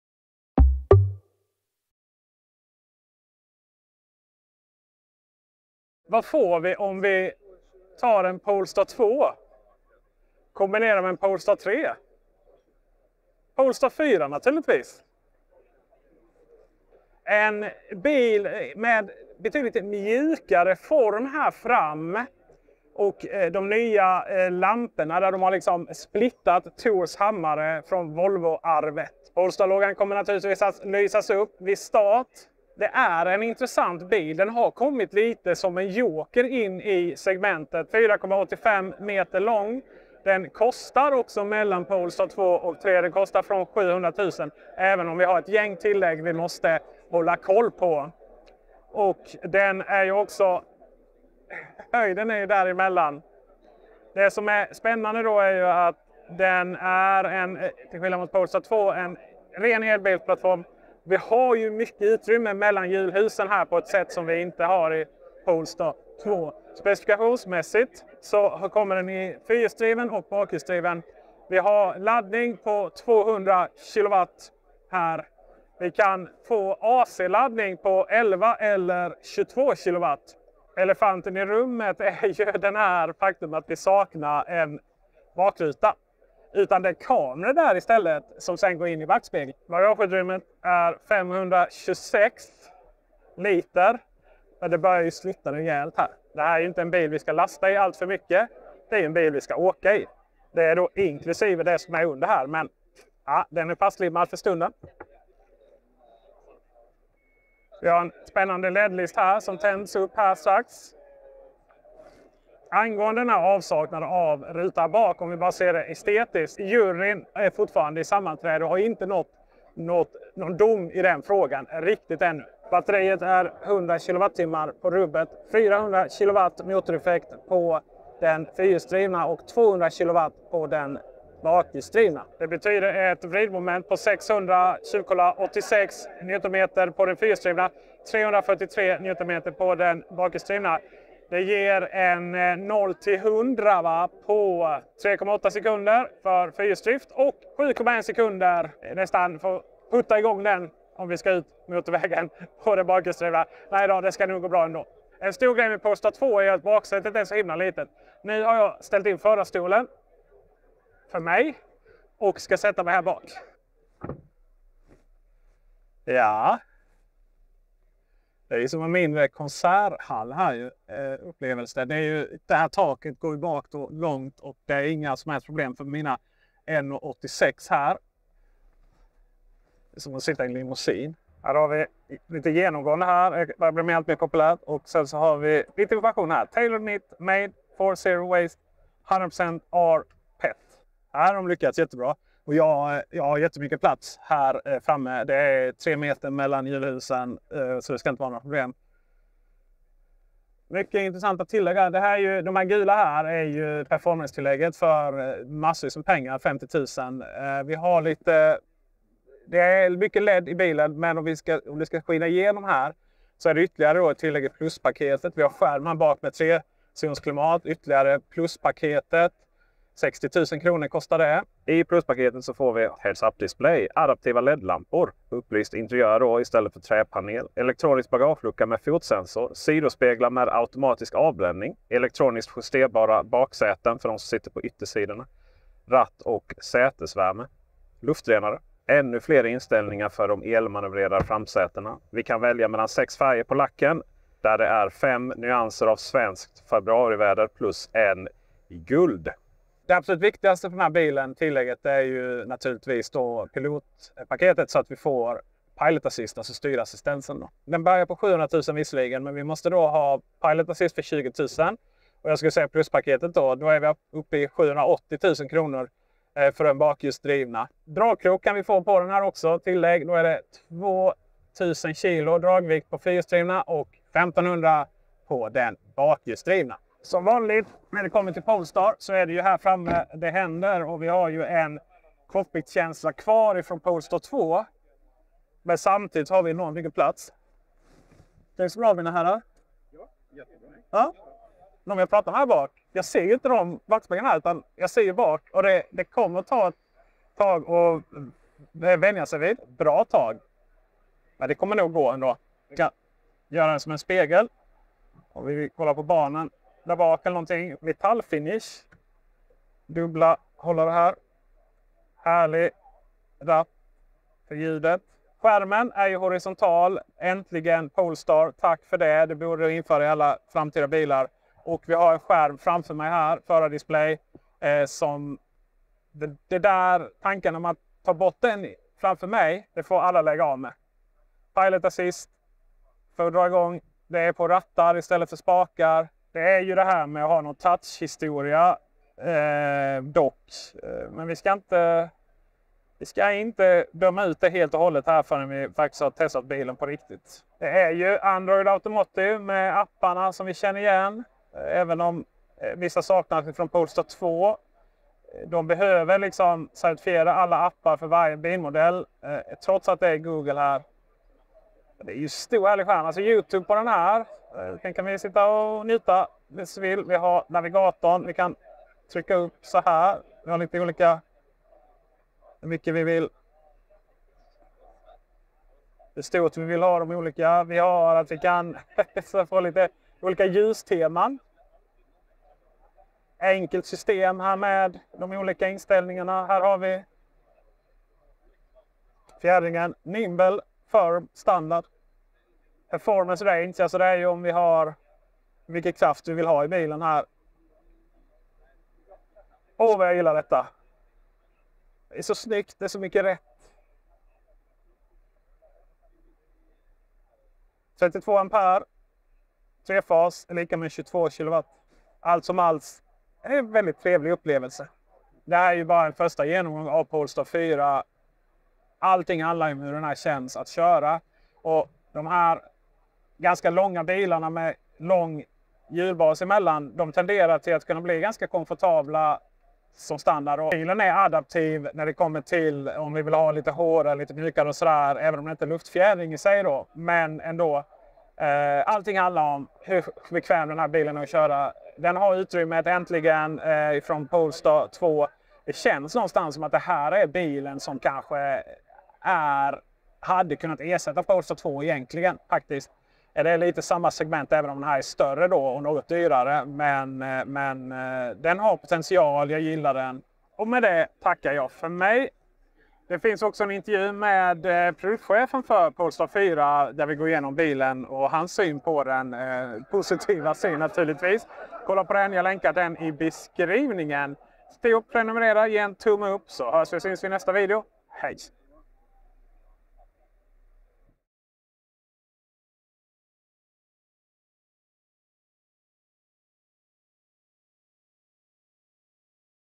Polestar 4 växer på en – så löser de saknad bakruta
Vi har varit så upptagna med att gå och vänta på Polestar 3 att vi inte lagt så mycket tid och energi på Polestar 4. Men det är en riktigt intressant bil som dock saknar bakruta. Men det fungerar även om det tar lite tid att vänja sig.

Trots att Polestar 4 ligger en bra bit söder om 5 meter så är den rejält tilltagen på insidan och vi får bra plats.

Intervjun med Ola Aldensjö, produktchef för Polestar 4, kommer snart.

Polestar 4 går att beställa nu och levereras i slutet av sommaren